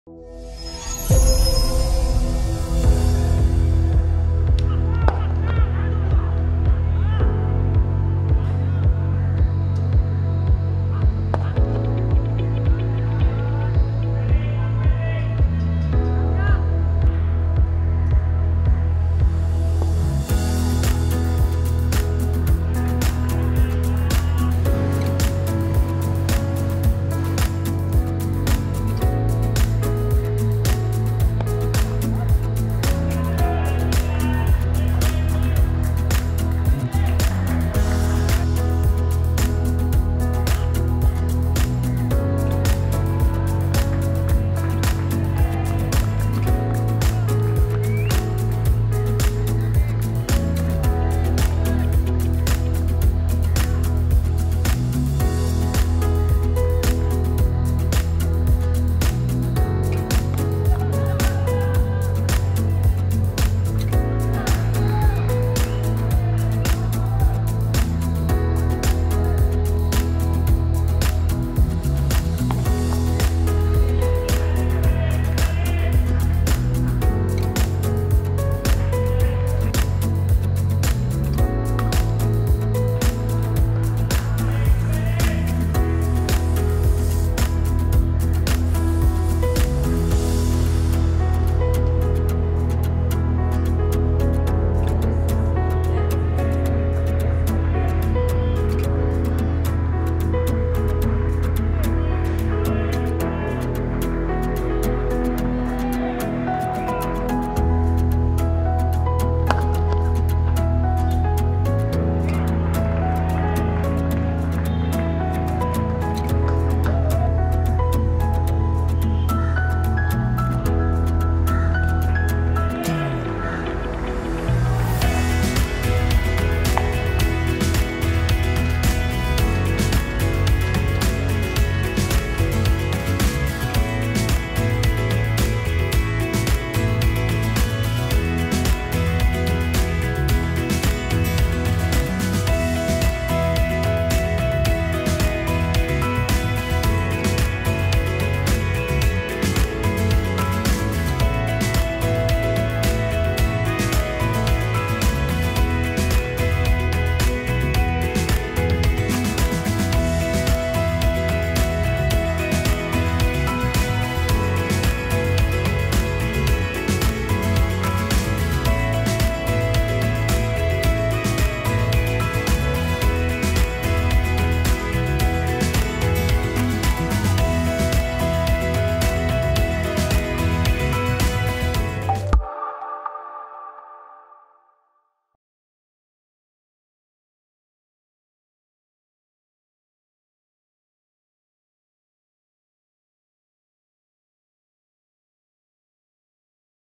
Sadeera Samarawickrama's 188 (243) vs CCC - SLC Major League Tournament 2022 - Final
Tamil Union C & AC Captain Sadeera Samarawickrama scored 188 runs off 243 deliveries in the SLC Major League Tournament 2022 – Final. The match was played on 17th - 20 of November at SSC Grounds, Colombo.